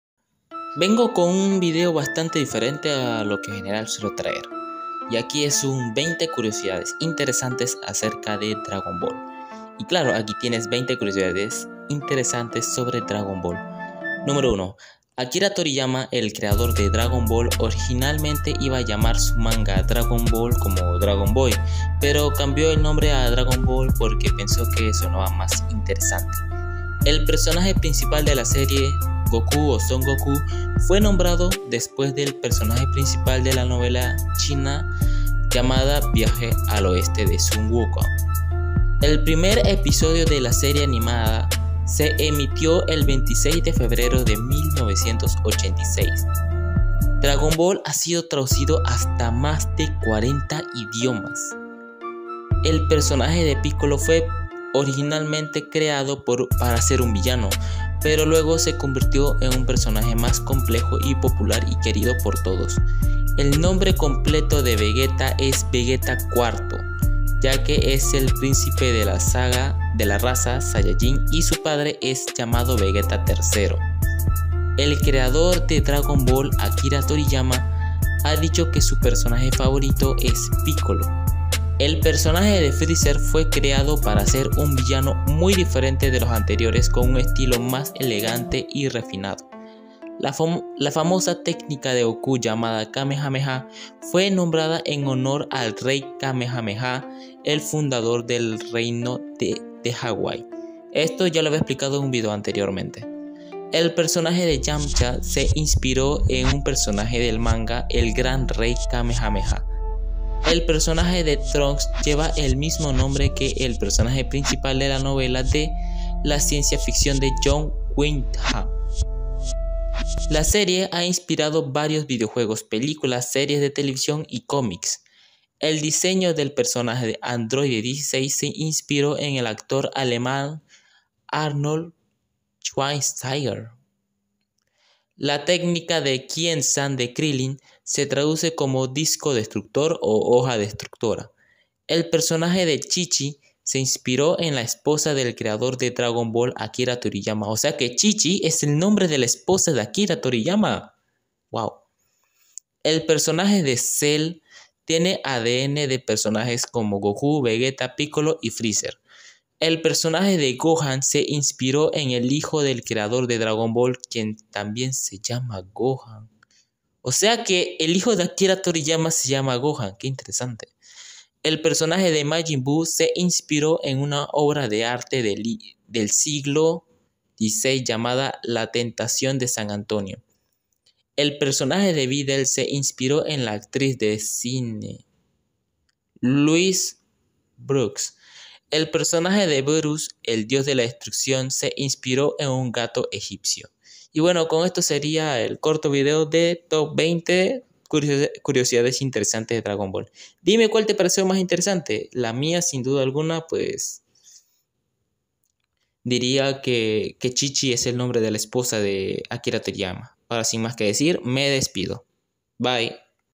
Curiosidades de dragon •|• 20 Curiosidades de dragon ball y goku
vengo con un video nuevo acerca de unas 20 curiosidades de dragon ball en un video corto sin mas para que ustedes conozcan algunas curiosidades de dragon ball, dragon ball z, dragon ball super y de goku al igual que de sus emblemáticos personajes dentro del anime y la trama de las sagas que ha tenido dragon ball a lo largo de los años